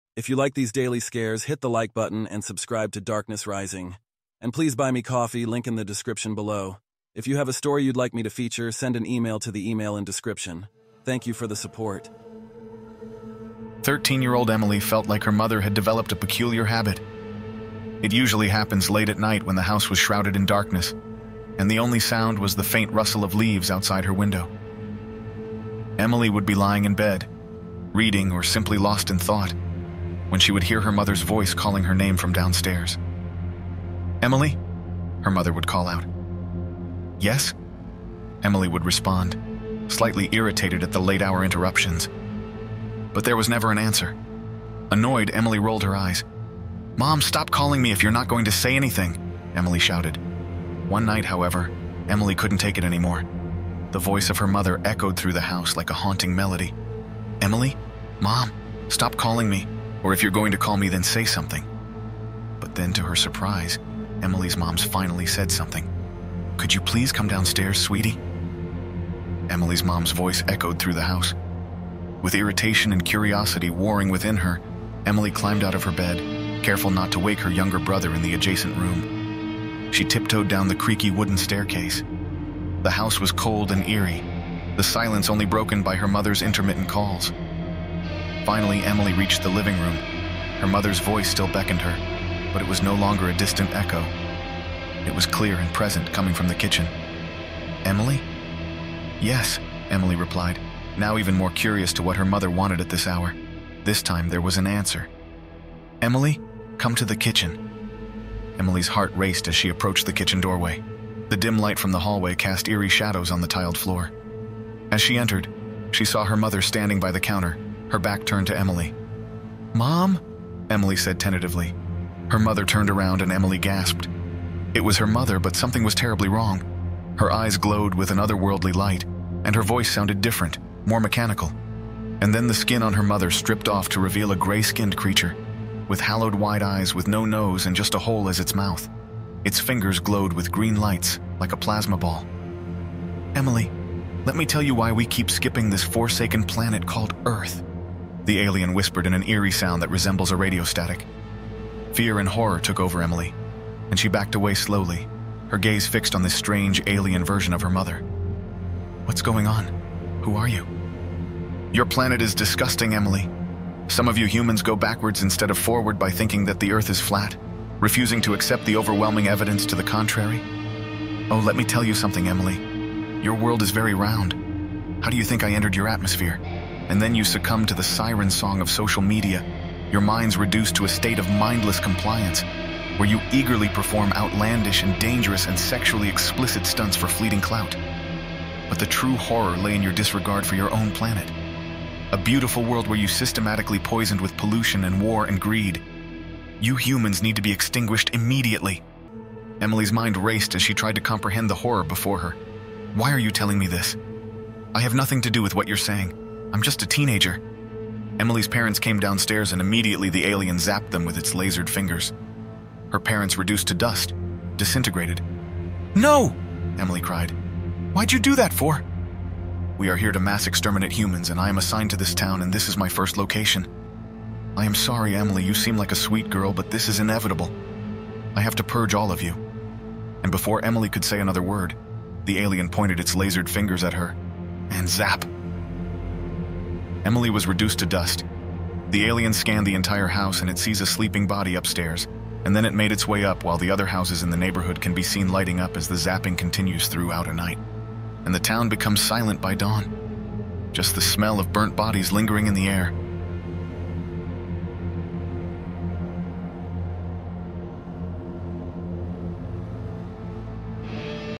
The Night Visitor - A Spine-Tingling Outworldly Horror Story
Please buy me coffee.

Emily's world is shattered when she discovers her mother's eerie calls hide a sinister secret—an alien takeover! 🛸 In a spine-chilling showdown, she battles to save her mom and humanity from extraterrestrial deception.